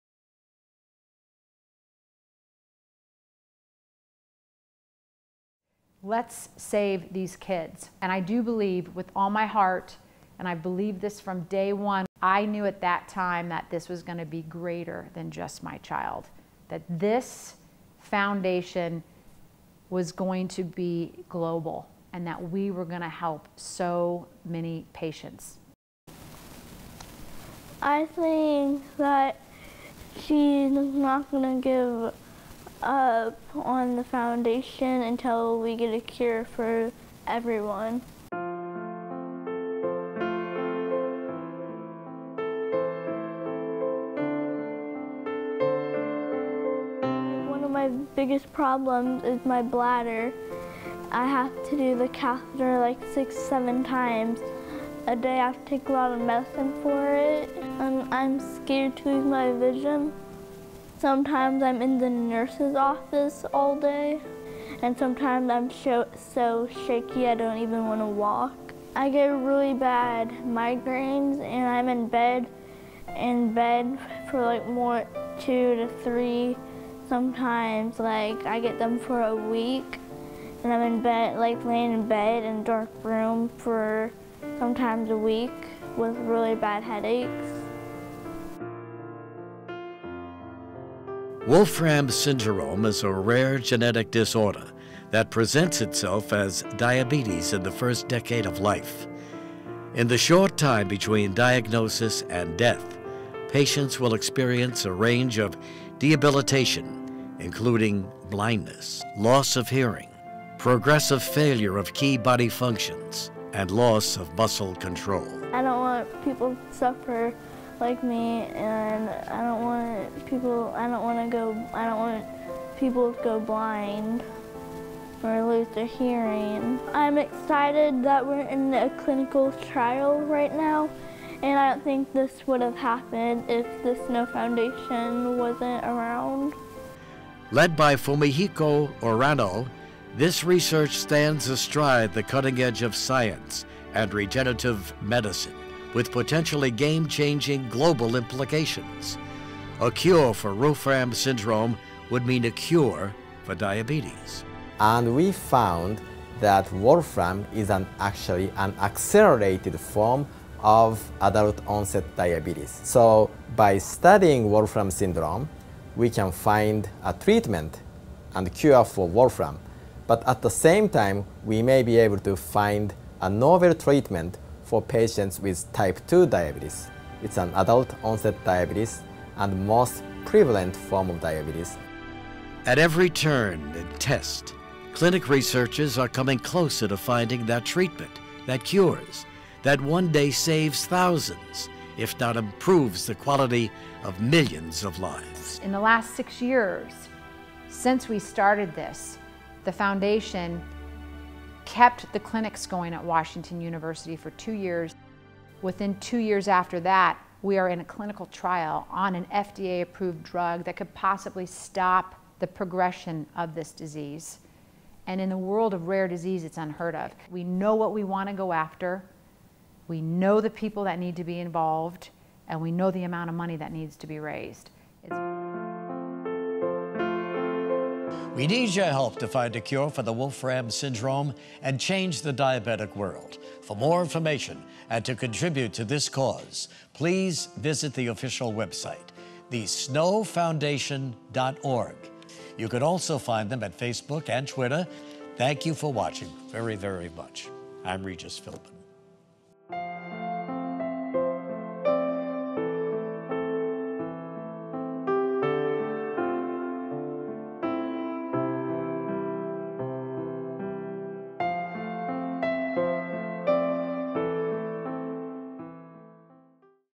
Let's Cure Raquel
We are on a mission to  help all children like Raquel Gebel.  She has Wolfram Syndrome, which is robbing her of her sight (almost gone now), hearing (just starting to loose), other organ problems will get worse, and it will rob her of her life before 30.  The Snow Foundation funds research for Wolfram Syndrome.  We want to inspire people to donate as much as they can.  Meet Raquel, who is a totally normal, beautiful, sweet, kind little girl right now.  No one could tell something is dreadfully wrong with her.  But soon that ends when she can no longer see anything.